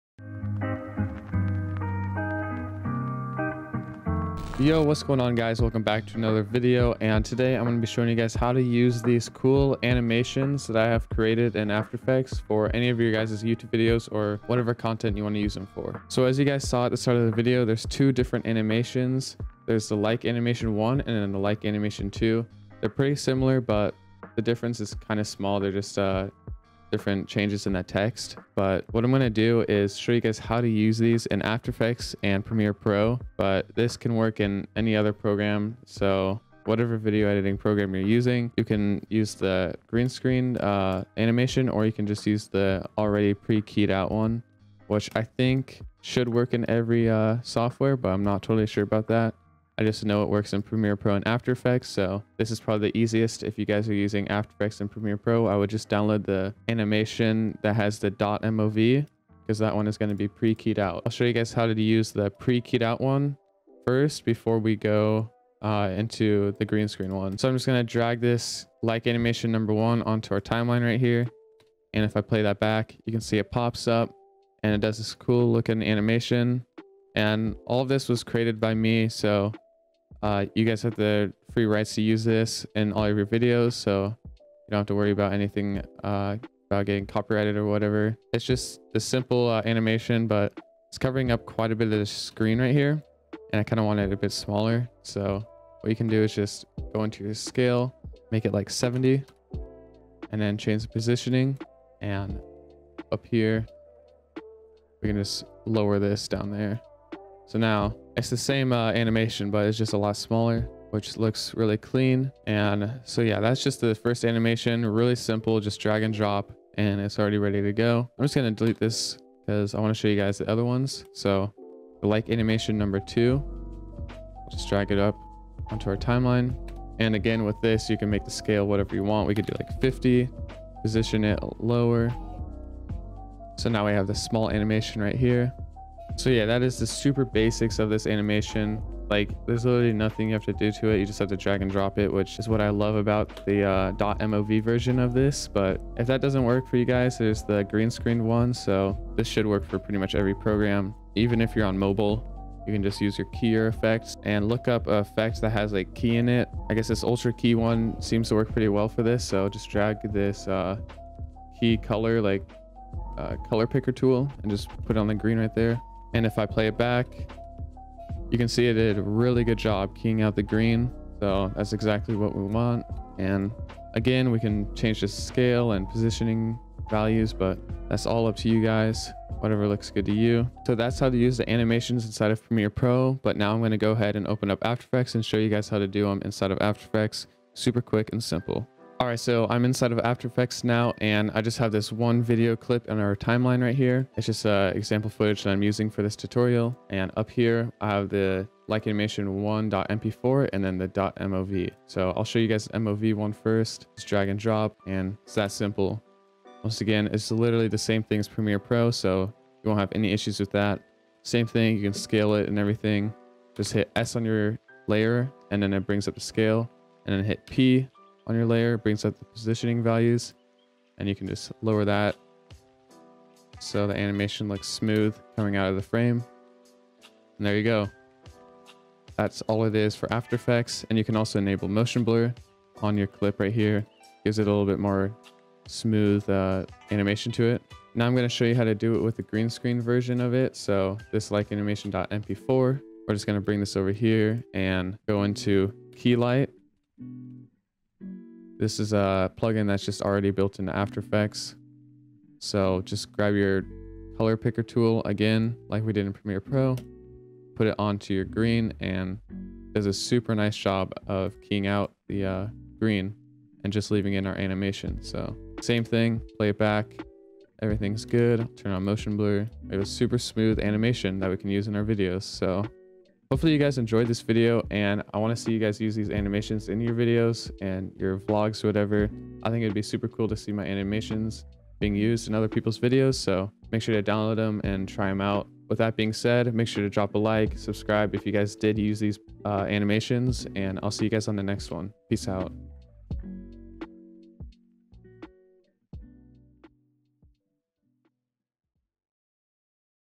How to use animation overlays like Adin Ross in any software!
In this video, I show you how to use these animation overlays I created for any of your videos! (After Effects, Premiere Pro, Final Cut Pro, etc.)